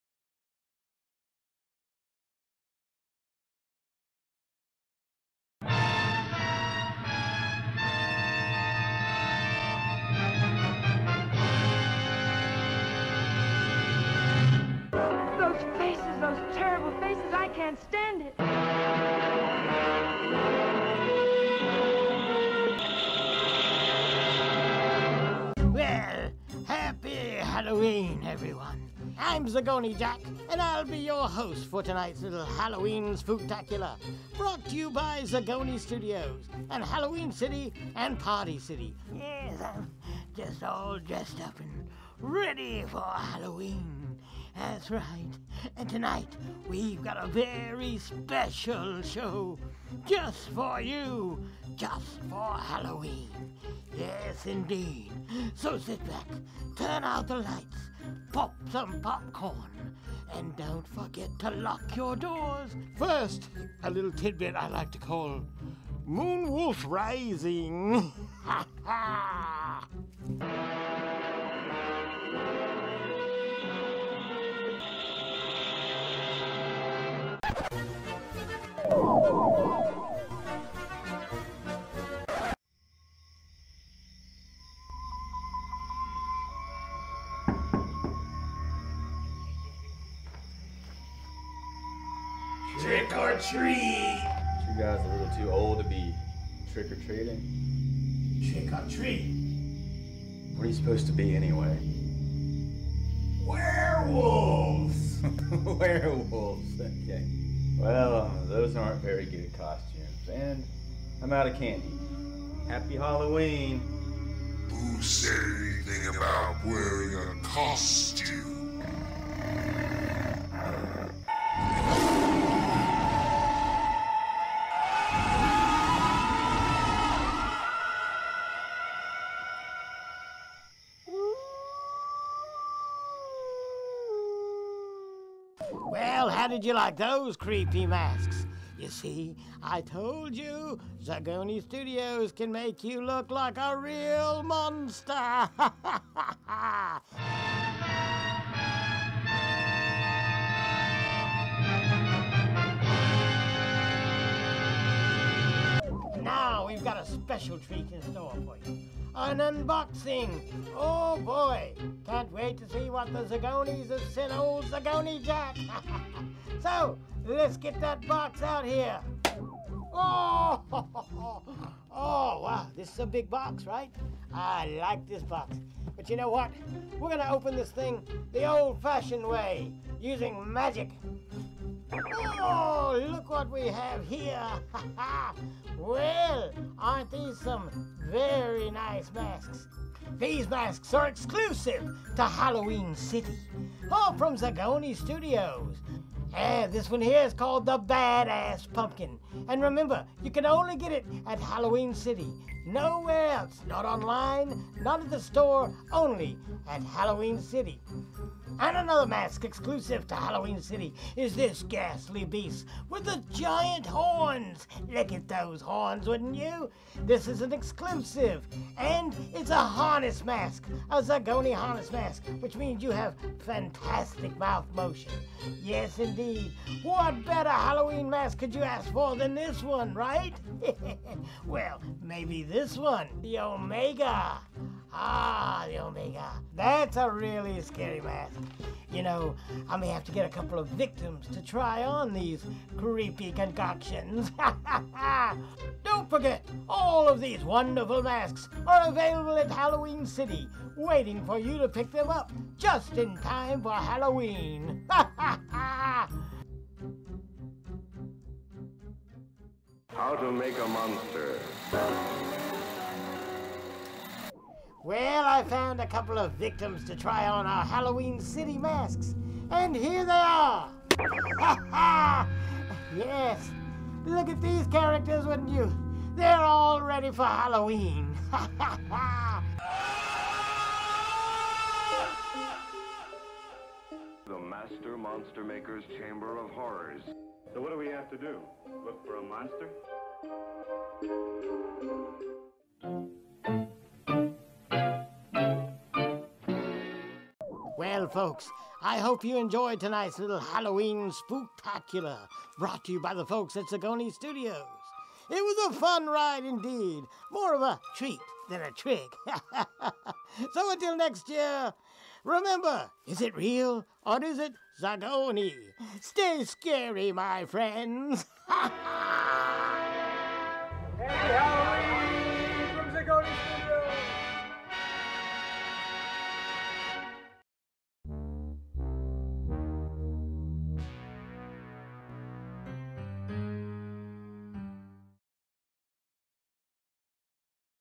A ZAGONE STUDIOS HALLOWEEN
From our friend Ron Mclellen and unboxing of the New Pumpkin Monster.